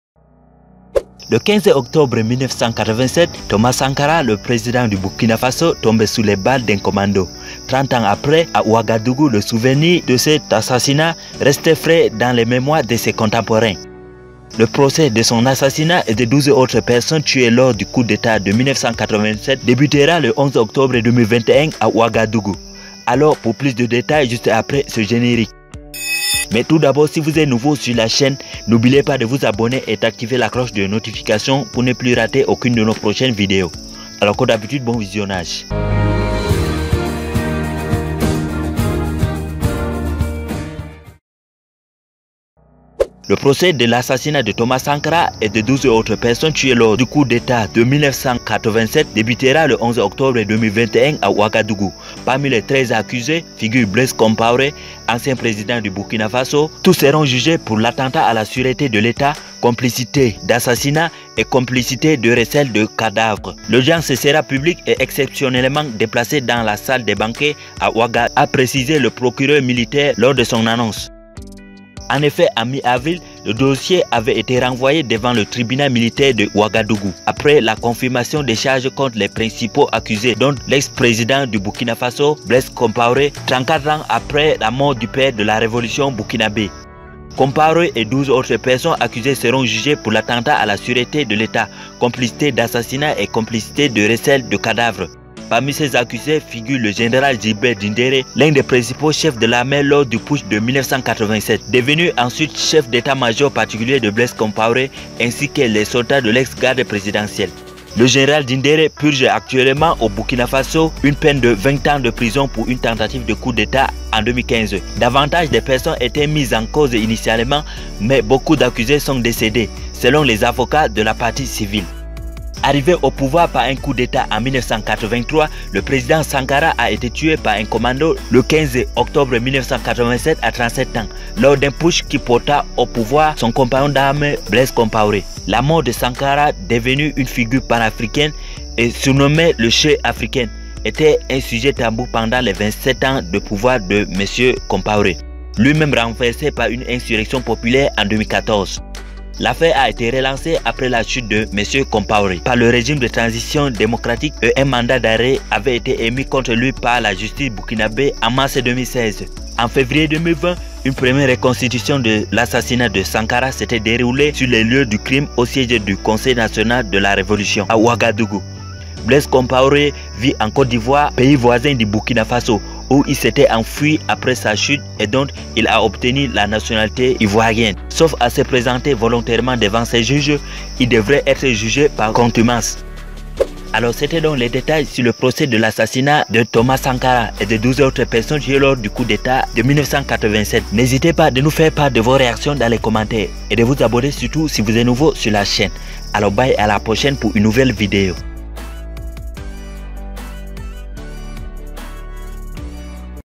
Le procès d'assassinat de Thomas Sankara I magazine du savoir
Le procès de l'assassinat de Thomas Sankara et des 12 autres personnes tuées lors du coup d'Etat de 1987 débutera le 11 octobre à Ouagadougou. Parmi les 13 accusés, figure Blaise Compaoré , ancien président burkinabé.   Ad Tous seront jugés pour "attentat à la sûreté de l'Etat", "complicité d'assassinat" et "complicité de recel de cadavres" . L'audience sera publique , et exceptionnellement déplacée dans la salle des banquets à Ouaga 2000 , a précisé le procureur militaire lors de son annonce.  Devenu un symbole et surnommé le "Che Africain" , Thomas Sankara a été tué lors d'un putsch qui mena Blaise Compaoré au pouvoir. Sa mort est restée taboue pendant les 27 années de présidence de son ancien compagnon d'armes...